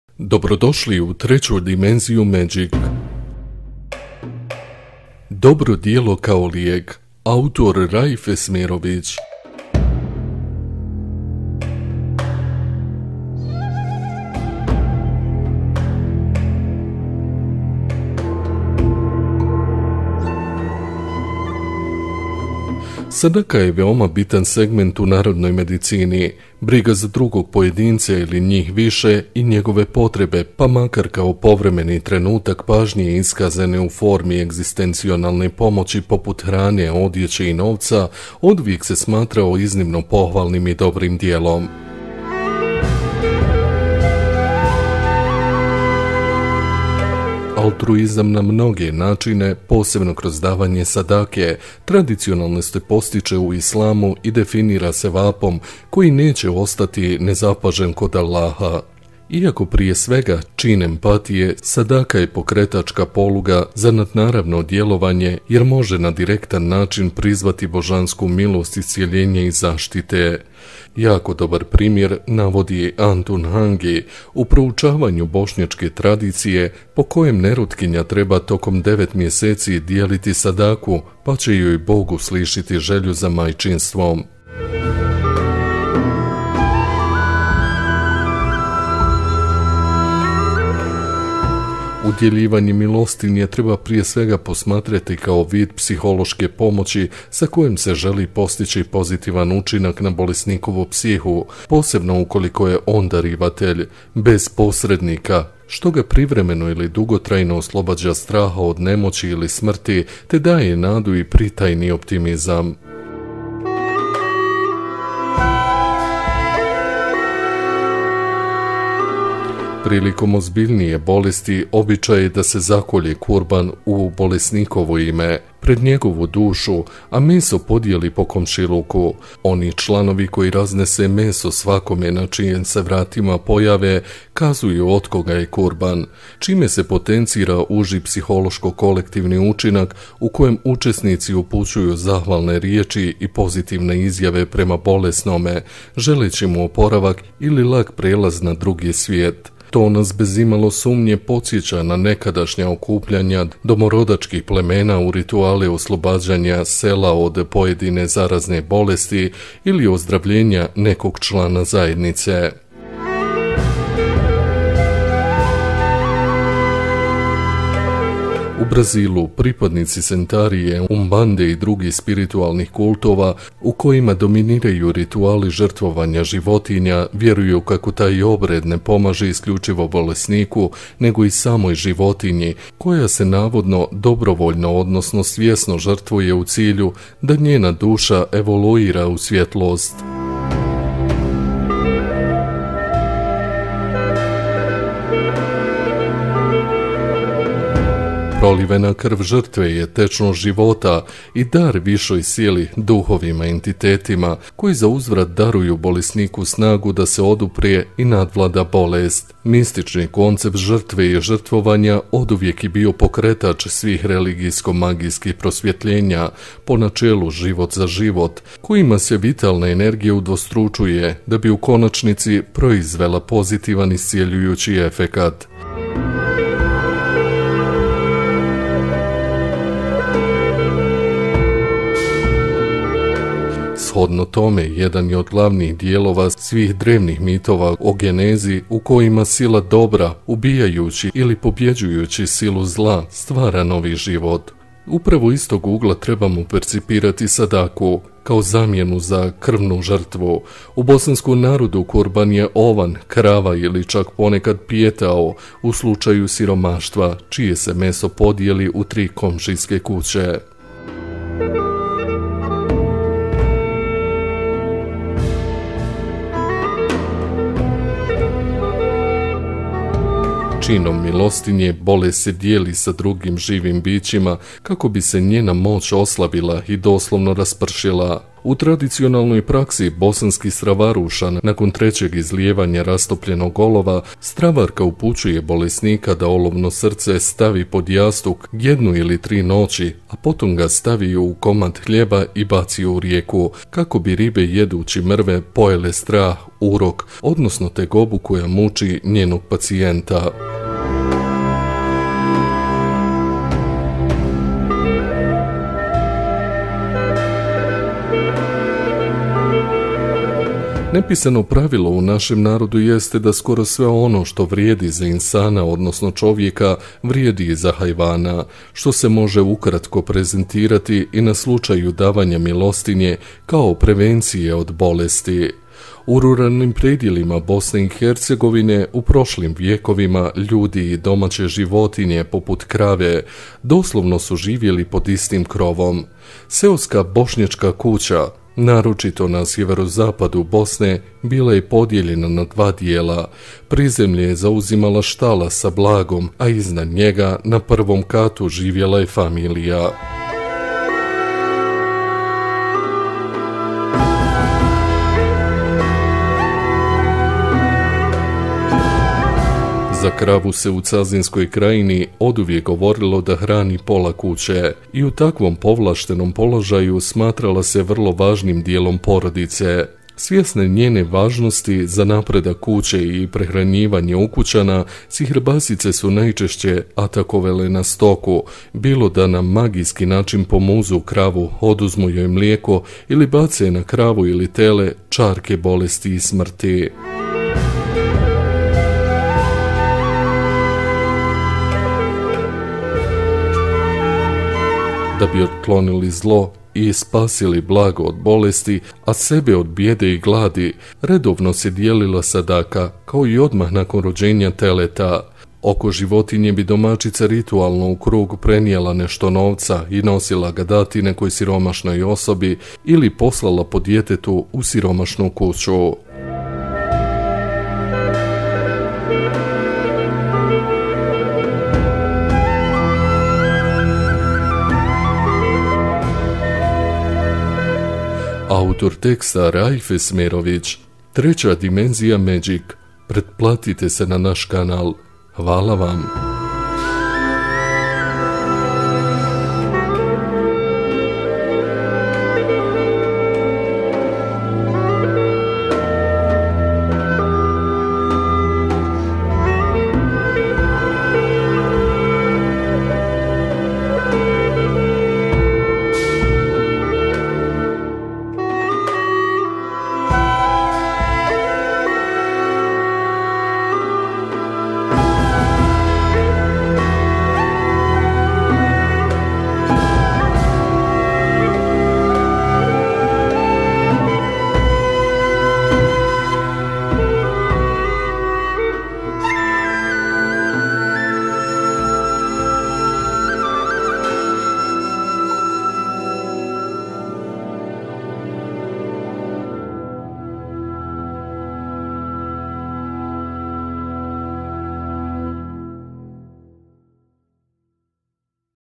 Dobro djelo kao lijek - SADAKA
tekst Raif ESMEROVIĆ ... Sadaka je veoma bitan segment u narodnoj medicini. Briga za drugog pojedinca, ili njih više, i njegove potrebe, pa makar kao povremeni trenutak pažnje iskazane u formi egzistencijalne pomoći poput hrane, odjeće ili novca oduvijek se smatrao iznimno pohvalnim i dobrim djelom.